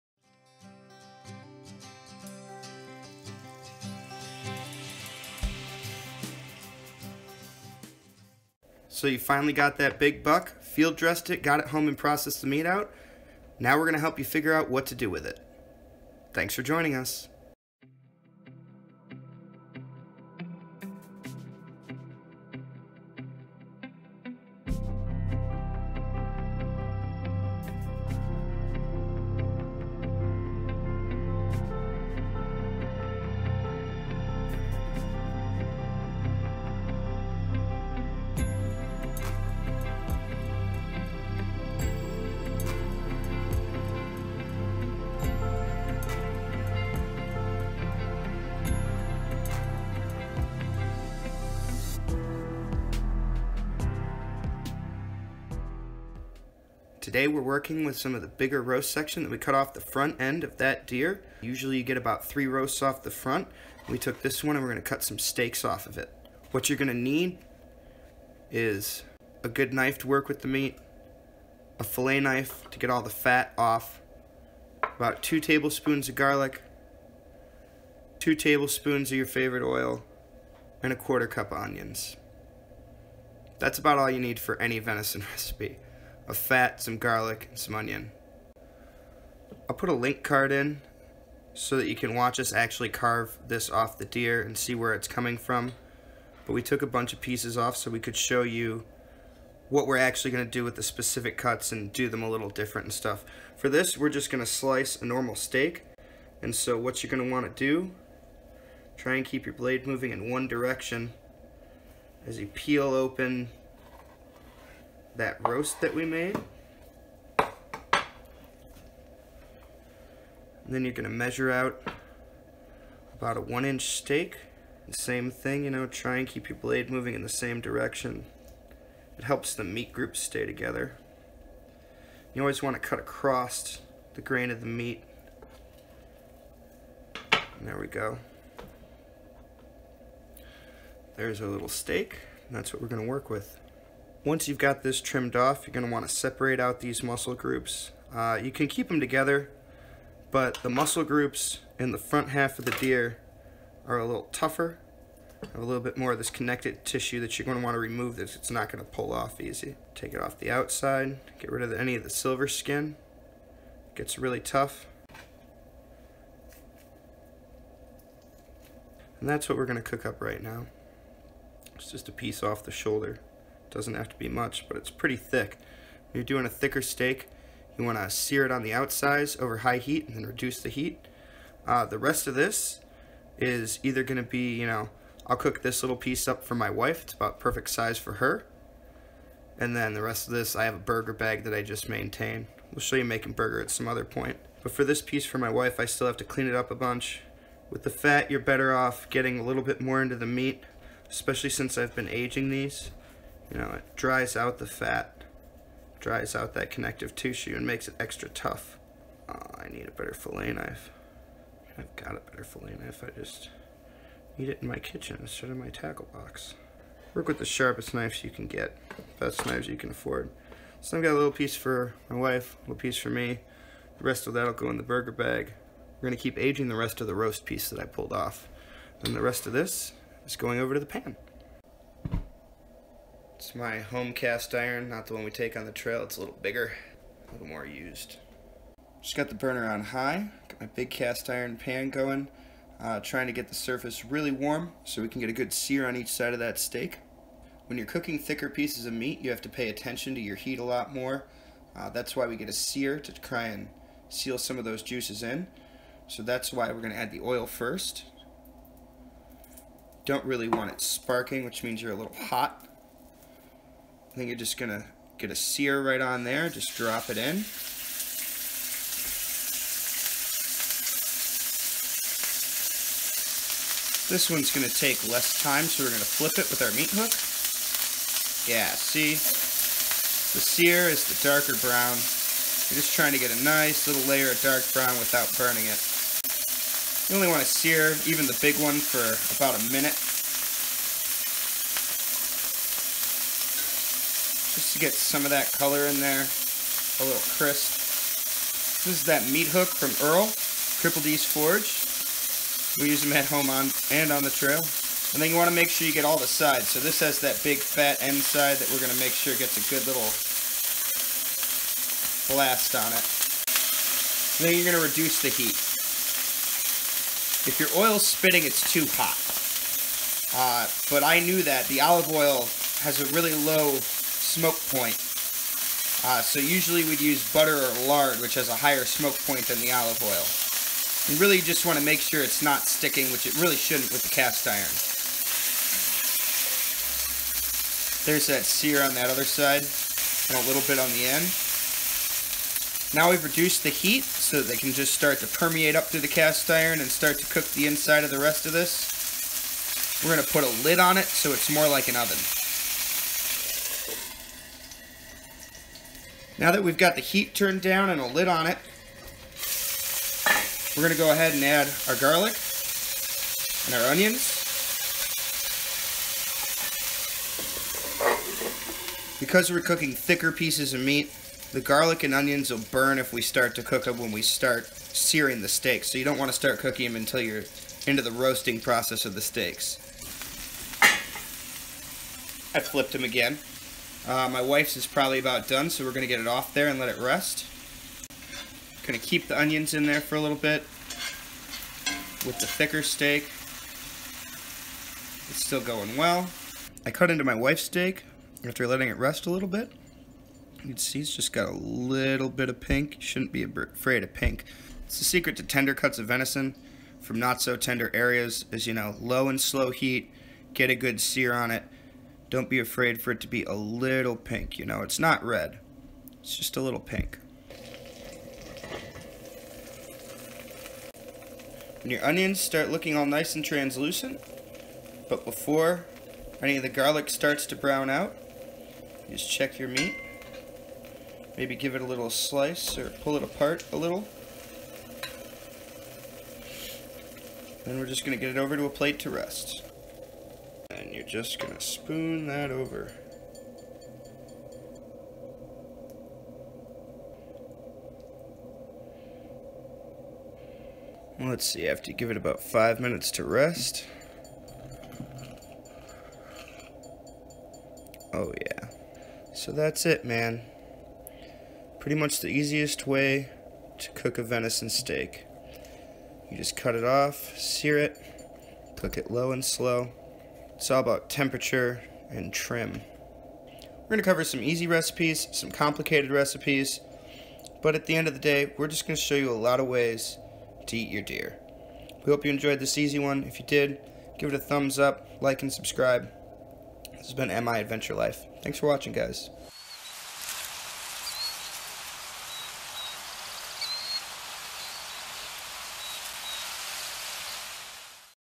Field to Table: Easy Venison Steak...
On the first cooking episode of Field to Table, we start with one of the shoulder roasts from our deer. Watch as we process a venison steak out of it to be fried up in a cast iron pan with some garlic and onions. It doesn't get much more simple than that, or more delicious. Click the links in the video to watch us harvest and process this deer from Field to Table.

Easy Venison Steak

1or2 four to eight ounce venison steaks
2tbs cooking oil
2tbs minced garlic
1/4 cup chopped white onion

Sear steak on all sides in oiled cast iron skillet on high heat (about 1min per side).
Reduce heat, add onions and garlic then cover.
Flip once (3 to 5 minutes) while stirring onions and garlic.
When onions are translucent and steak is to desired internal temperature, remove from heat and allow to rest for 5 minutes.
Cover in onions and garlic from pan.

Logo: @BIT_Spookery

Opening/Closing Graphics: @This_Show